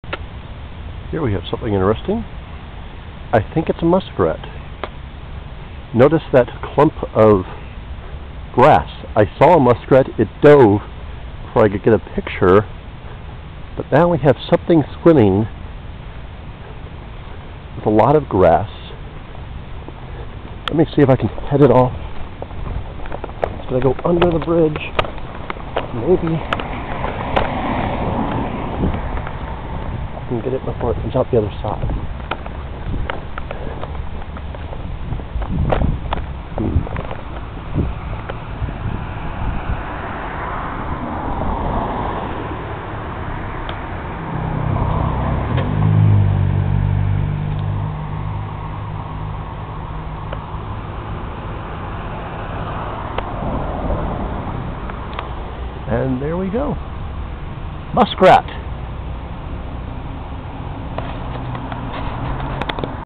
Muskrat Builder 1
I spotted a muskrat in the East Lagoon on the campus of Northern Illinois University.  Actually, I spotted it heading up the stream that feeds into the pond.  Turns out it was gathering some plant material, no doubt for its nest.  I followed it a pretty fair distance, as you can see in these two videos.